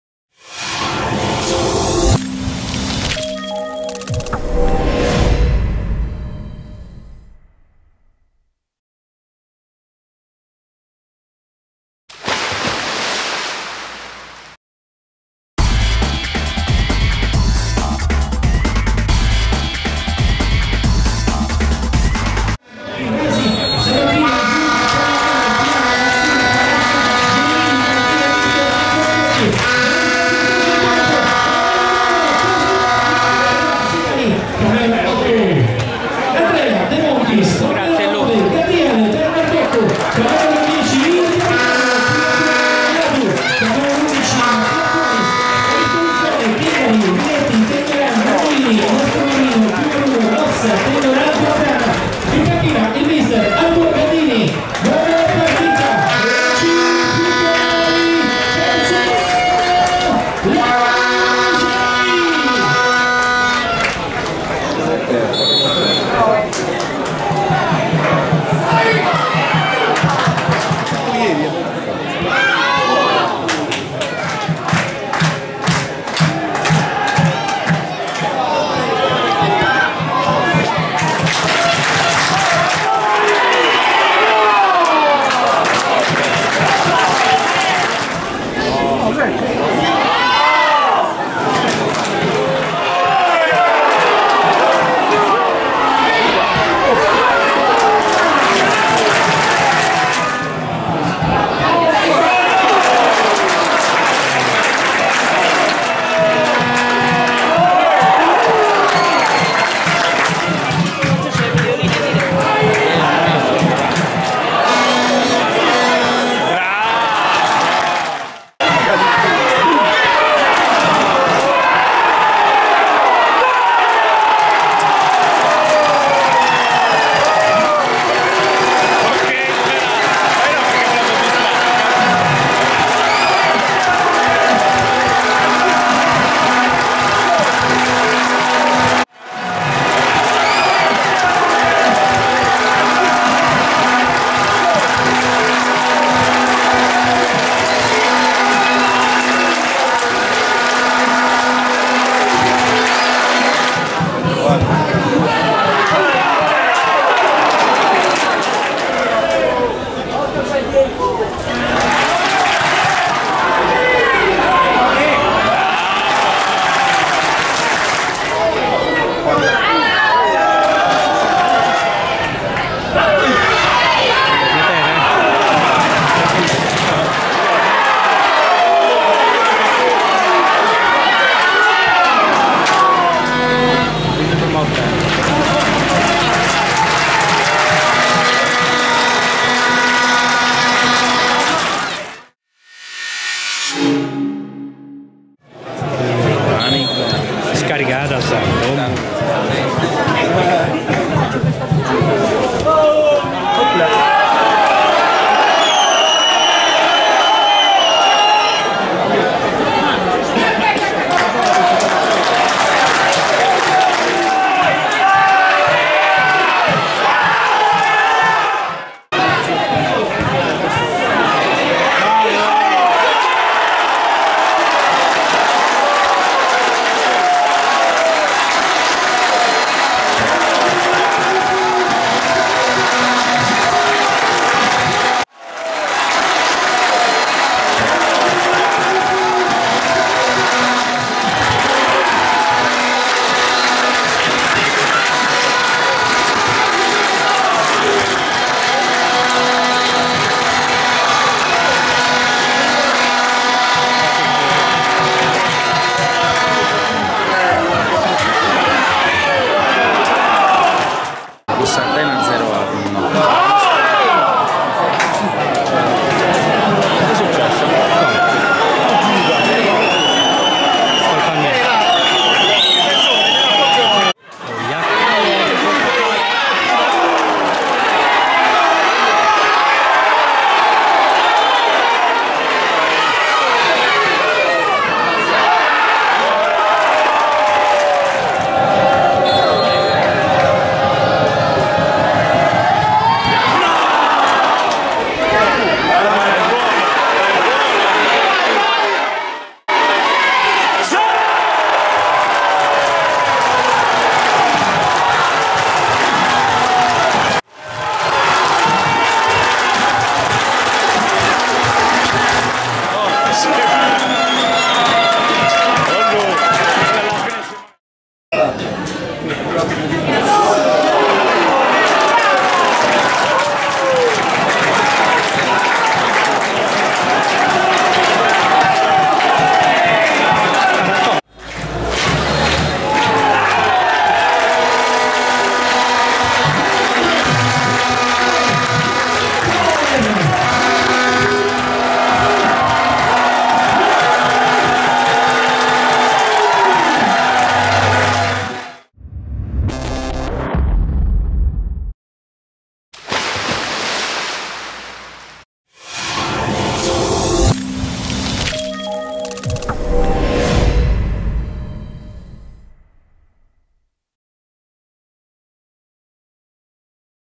ASD Lanusei Calcio 3 - Monterosi FC 1 - Highlights
Marcatori: 33′ Bernardotto (L), 62′ Floris (L), 66′ Nohman (M), 87′ Quatrana (L)
31° Giornata di Campionato di Serie D - Girone G
Stagione 2018/19
Stadio Lixius 17/03/2019
Foto Giorgio Melis
Riprese Video e Montaggio Sardegna Punto Radio